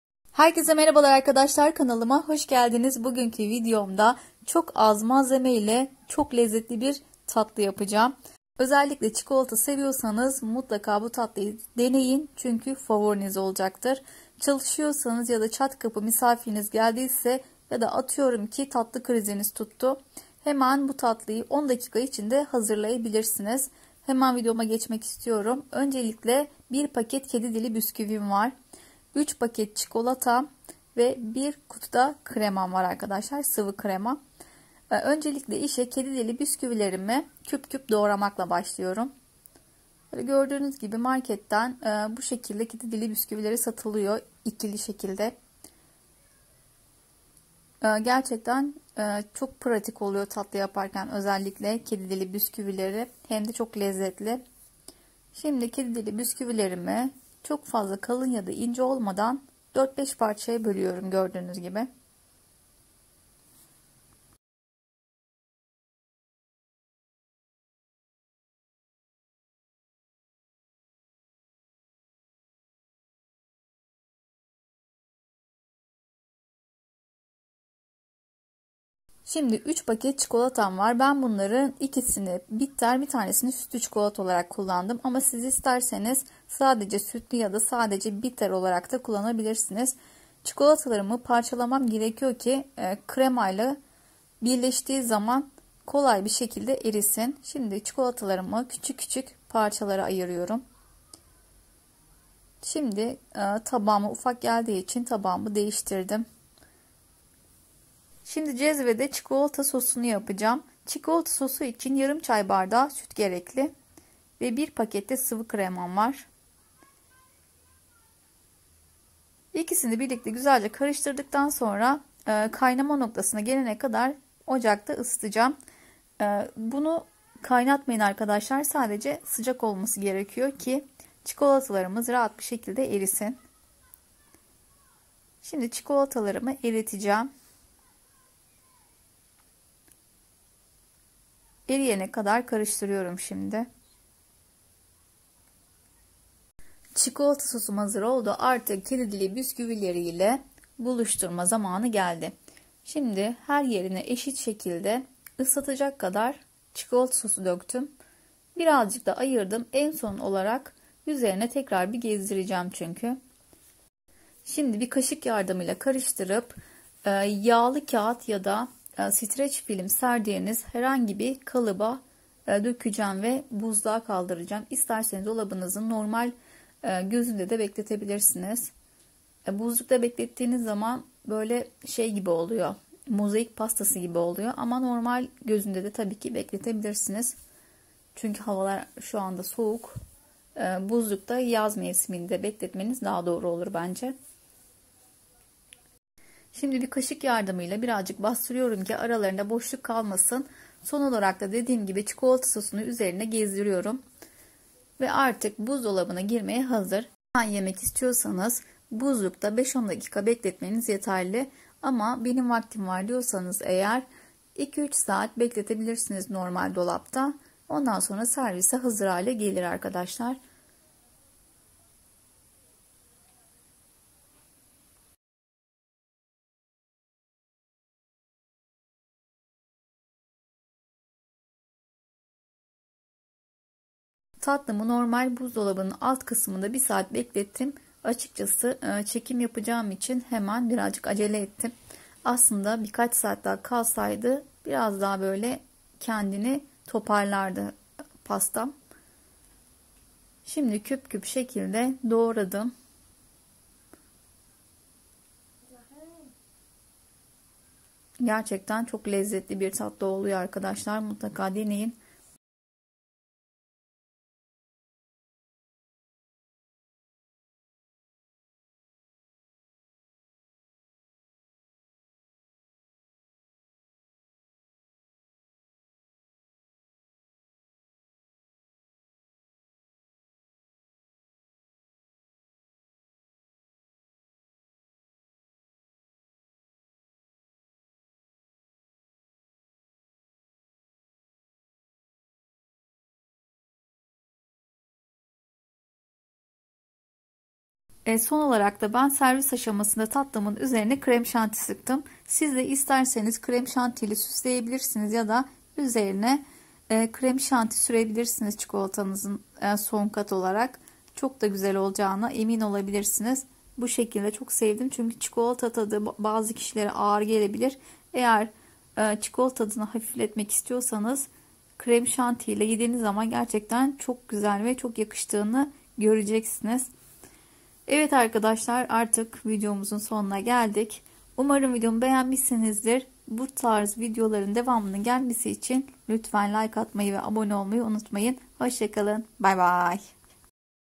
ÇİKOLATAYA DOYACAKSINIZ!!! SADECE 10 DAKİKADA ÇOK KOLAY BİR KEDİDİLİ PASTA YAPIN!
Merhaba, arkadaslar vaktiniz yoksa yada tatlı kriziniz tuttuysa cok kısa, sürede yapacagınız inanılmaz lezzetli bir tatlı tarifim var
    ÇİKOLATALI KEDİ DİLİ PASTA
  1 PAKET KEDİ DİLİ BİSKÜVİ
   3 PAKET ÇİKOLATA
   1 PAKET SIVI KREMA
    YARIM ÇAY BARDAĞI SÜT